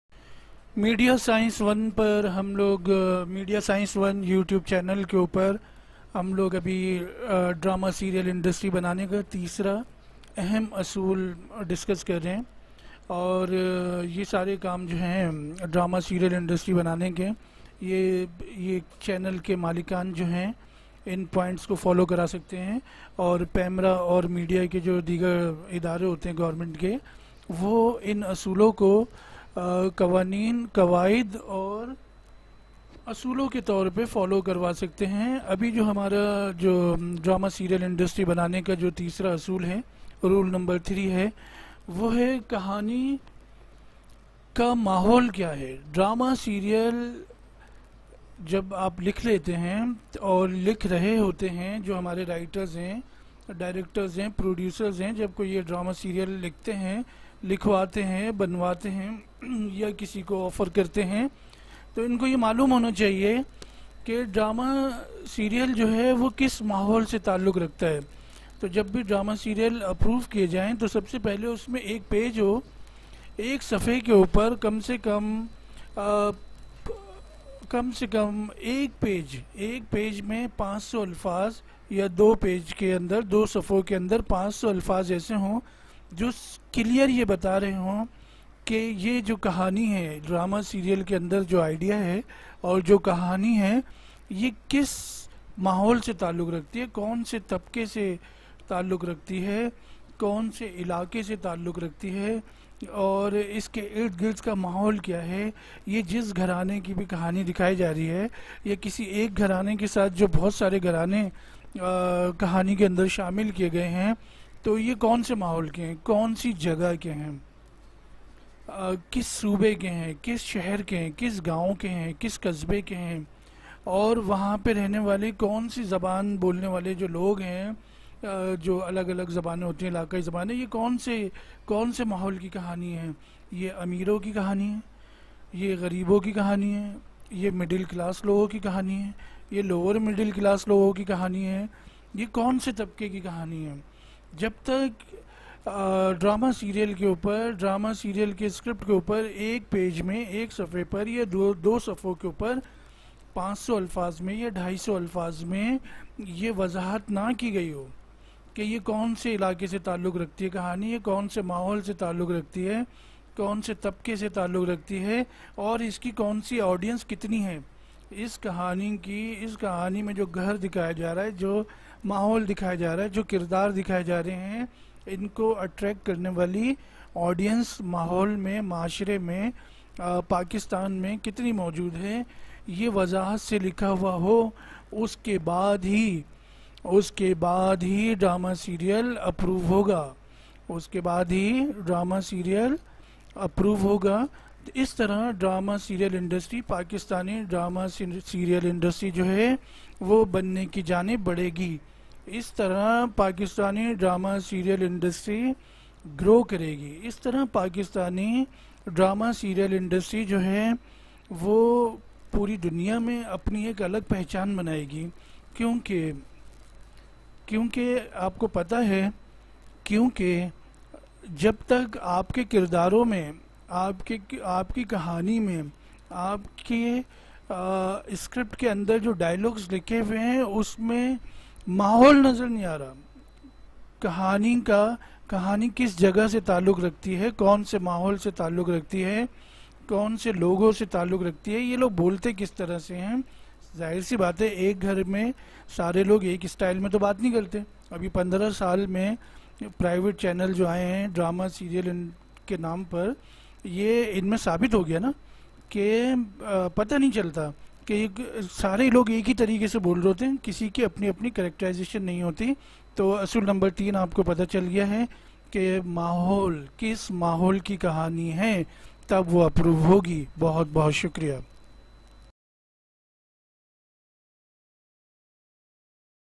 RULE#3-DRAMA-SERIAL INDUSTRY BANAANI HAY.
MEDIA SCIENCE ONE K DOSTOO-RULE#3 AAPKO SEEKHA OR SAMJHA RAHA HAY.DRAMA SERIAL K SCRIPT MAIN ENVIRONMENT-MAAHOL-
JAGA-ILAAQ-IRD GIRD KA MAASHRA-KION ZAROORI HAY.?
PEMRA OR DEEGAR MEDIA K IDAARAY RULE#3 KO ASOOL-QAWAAED-
QAWANEEN K TOR PAR RAAIJ KARAAE.JISKI WAJAH SE PAKISTANI DRAMA SERIAL INDUSTRY BANNAY KI JANIB APNA TEESRA QADAM BARHAA
DEGI.DUNYA KI DEEGAR MUMAALIK PAKISTANI DRAMA SERIAL KI SAHEE MAANI MAIN JO PEHCHAAN HAY US SE AAGAH HONA SHOORO HOJAAEGI.SHUKRIYA
WRITER/DIRECTOR
shakilmallick